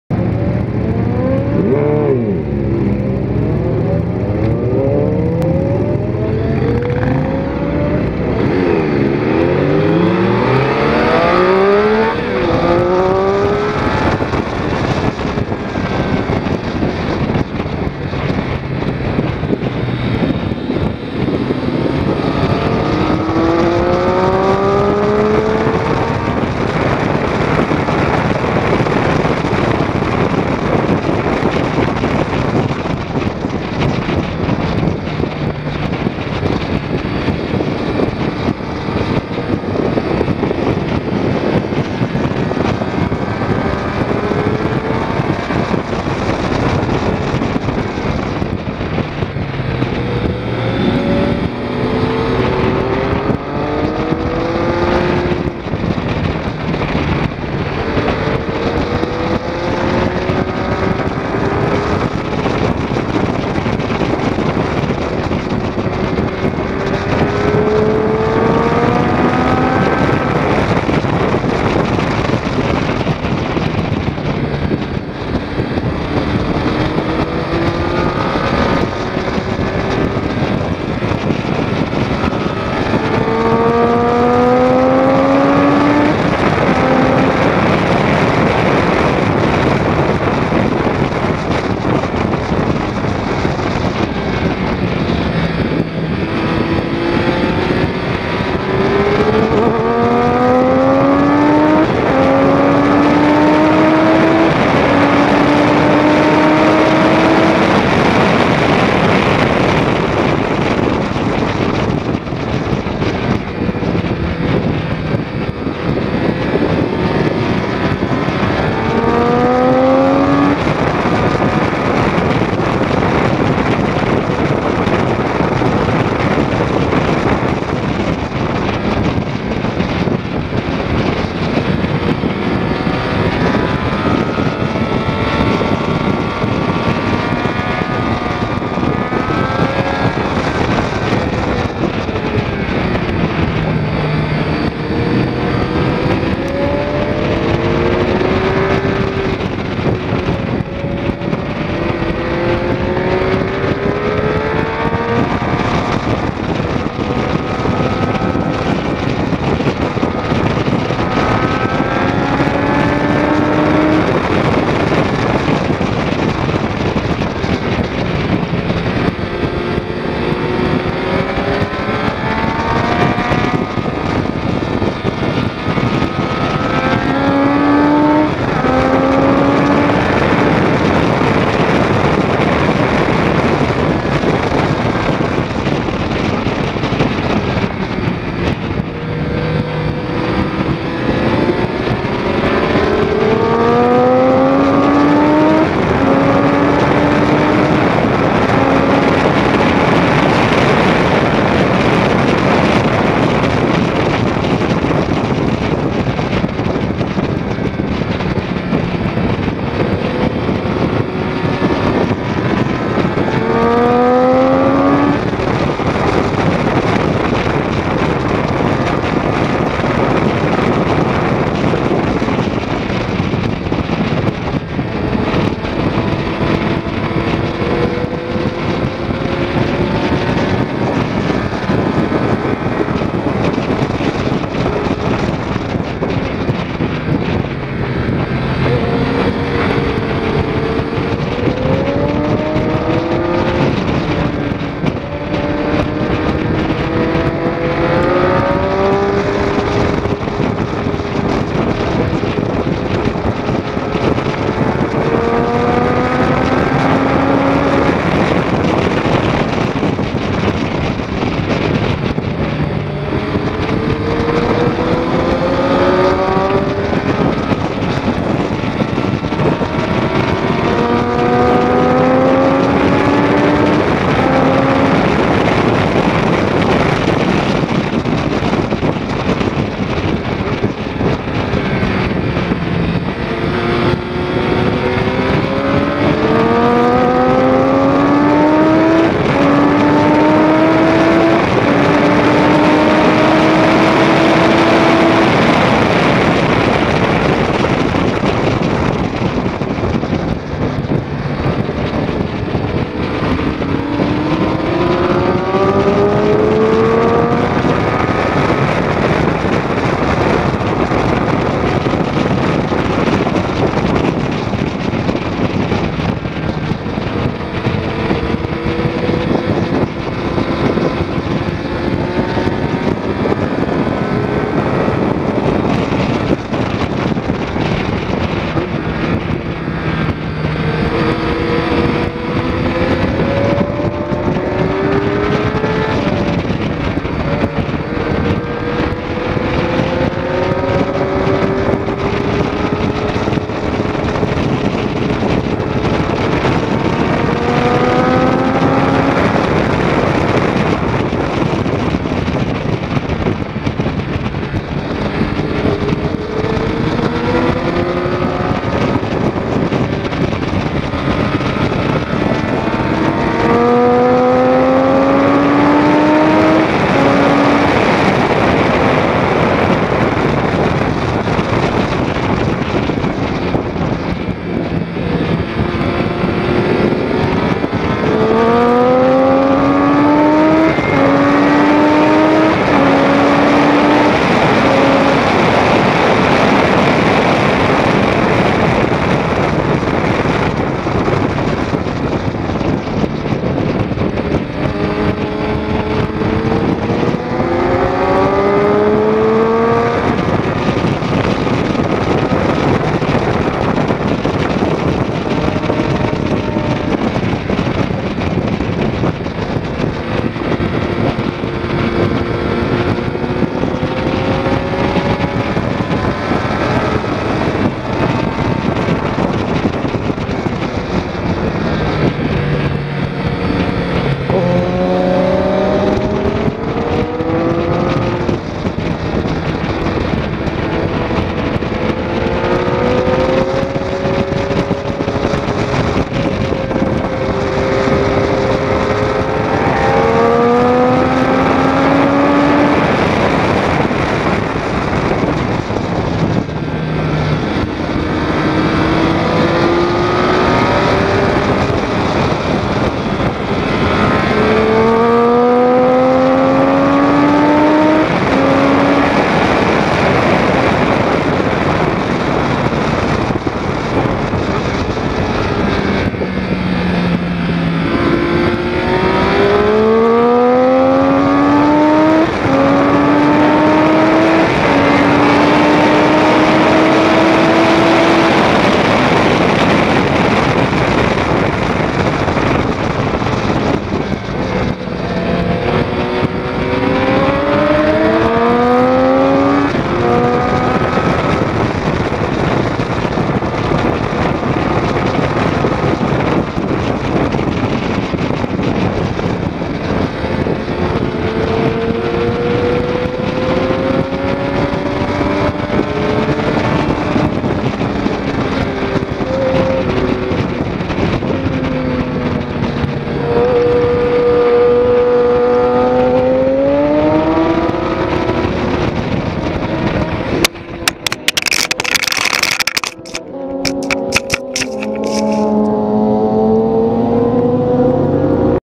Blackhawk Farms Track Day
MotoVid.com Track Day
June 8th 2013
2006 Triumph Daytona 675

First session in intermediate group. Lost my camera halfway through the session.